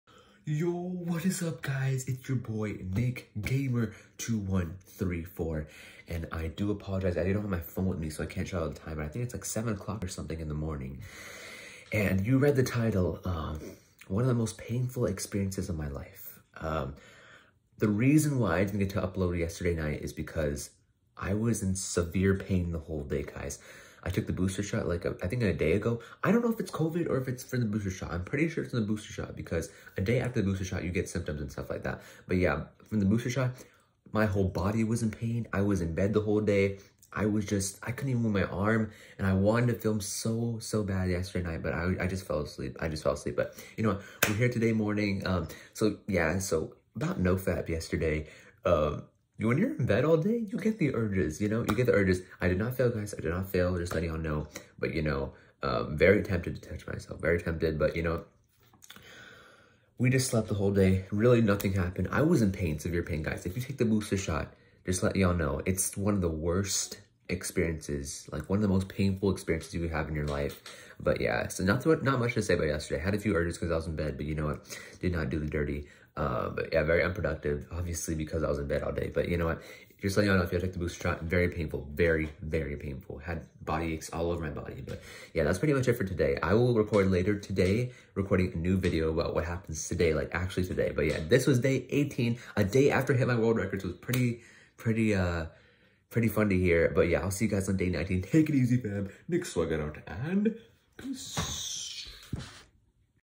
(One of the Most PAINFUL Experiences in my LIFE!!) - NoFAP Day 18
-I was in pain all day
-I was sleeping for 75% of the day
-Had a few urges in the morning, but yo boy NICKSWAGGER fights it off like its NOTHING